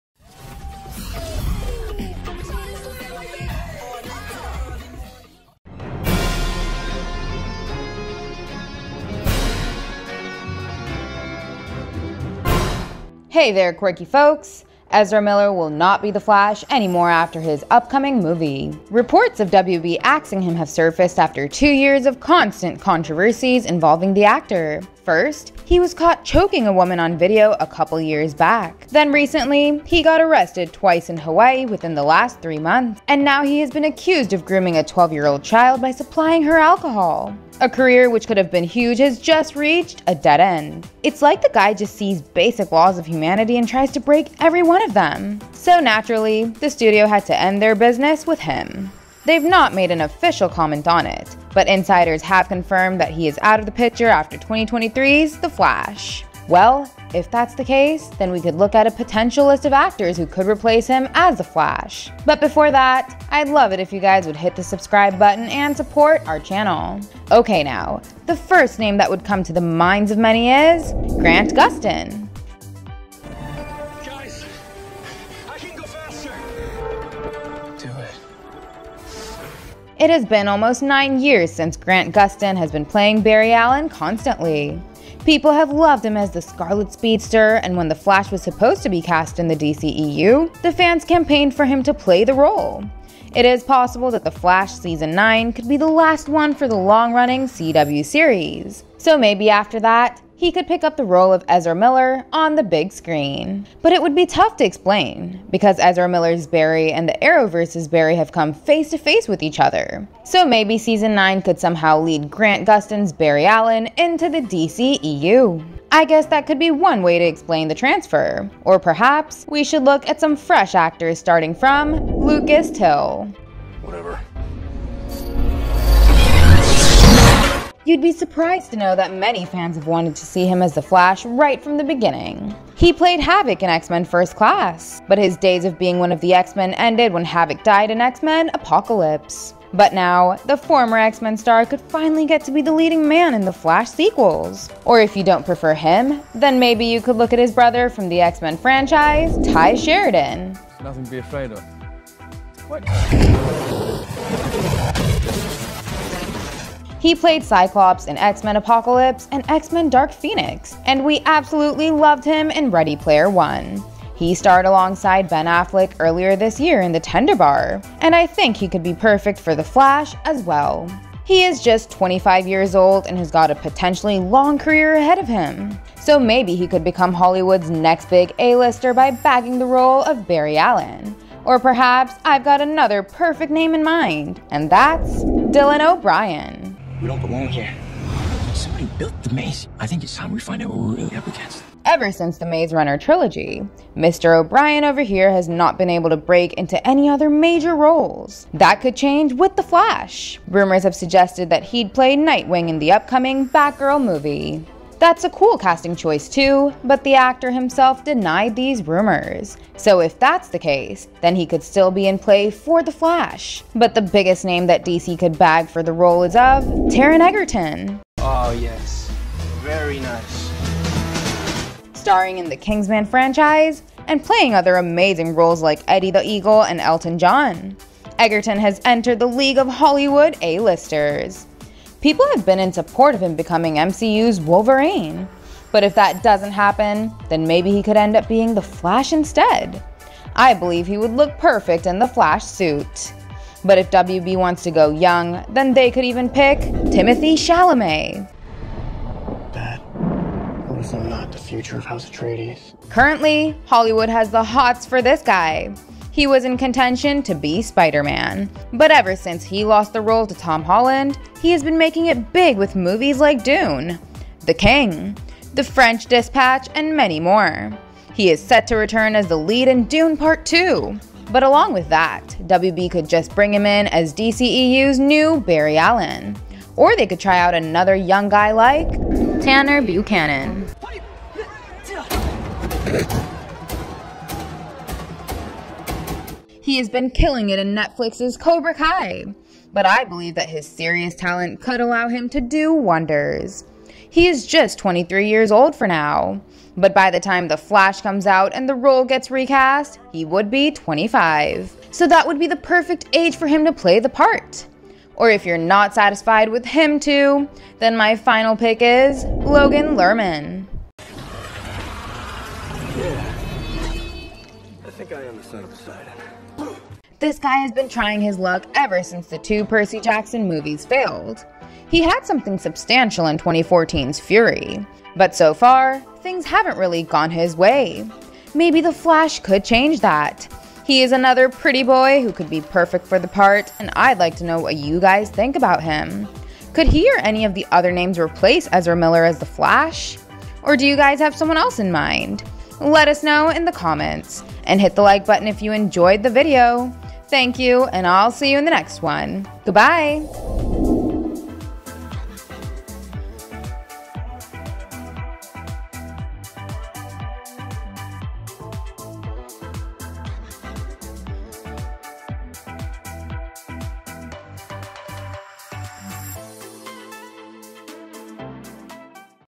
8 Actors Who Could Play The Flash After Ezra Miller
Ezra Miller will not be The Flash anymore after his upcoming movie. Reports of WB axing him have surfaced after 2 years of constant controversies involving the actor. First, he was caught choking a woman on video a couple years back. Then recently, he got arrested twice in Hawaii within the last 3 months. And now he has been accused of grooming a 12-year-old child by supplying her alcohol.

A career that could have been huge has just reached a dead end as he would obviously be out of Fantastic Beasts 4 (if that movie ever happens). It’s like the guy just sees the basic laws of humanity and tries to break every one of them. So naturally, the studio had to end their business with him. That means a new guy will come in. So, here are some actors who could replace Ezra Miller as the Flash in the DCEU.